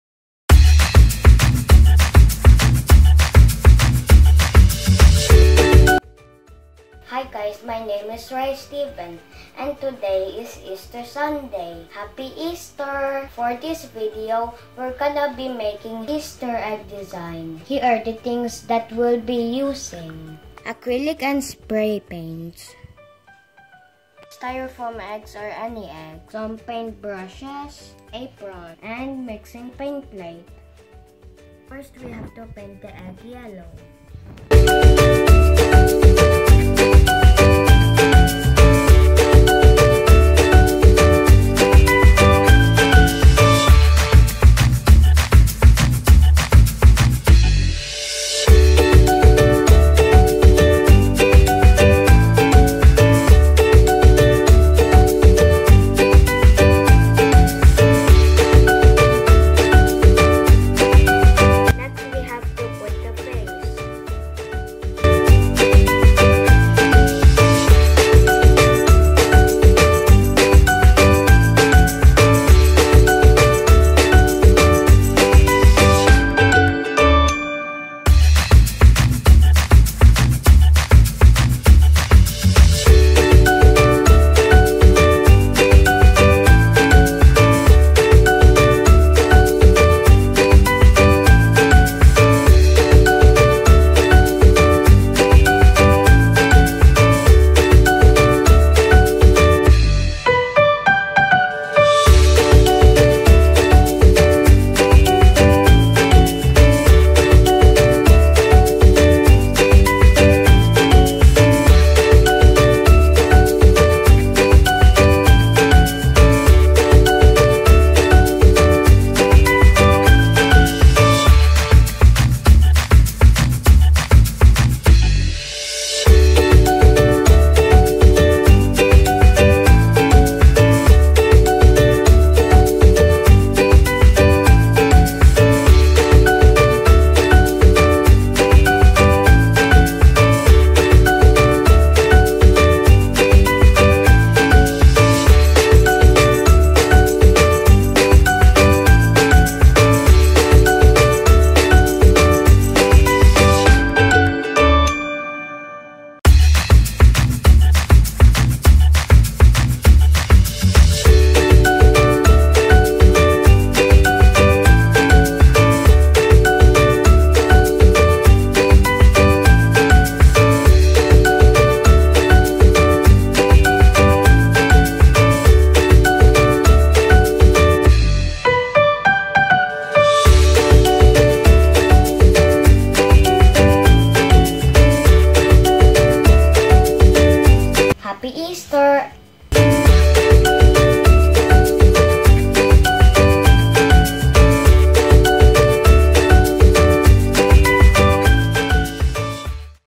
Celebrating Easter Egg Sunday - Painting Activity
MAPEH Q3; Values Q3
Painting Activity
Peniel Homeschool Doha
Rye Steven QP
Grade-1